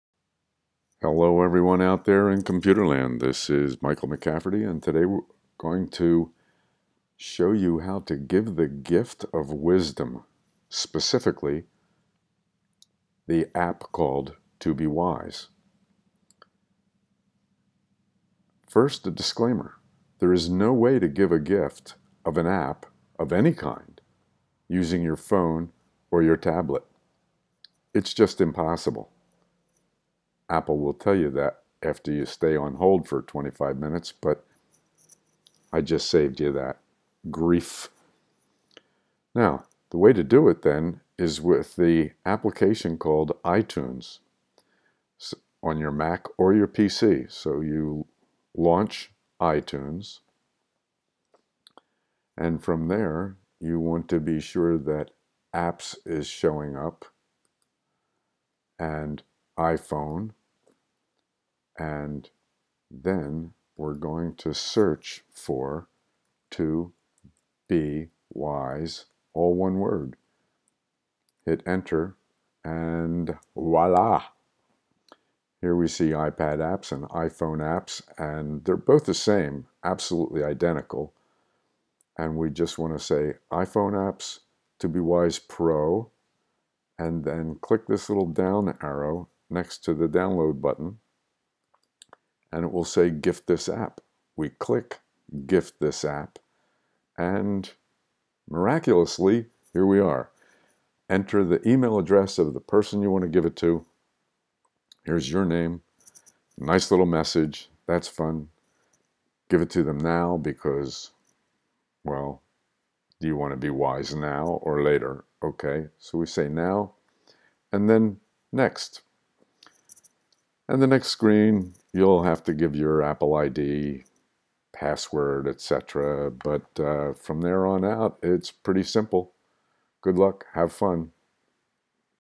ToBeWise Gifting
How to give the gift of Wisdom, in the form of the iPhone / iPad app called ToBeWise.
First of all: Apps can NOT be gifted using a mobile device, only a computer such as your PC or Mac. This example shows the Mac.
Whichever computer you use, you need to launch ITUNES, then search for the app tobewise (all one word) and then enter. Next to the app download button, click the down arrow, and there it is: Gift the app.  The rest is easy!  Enjoy!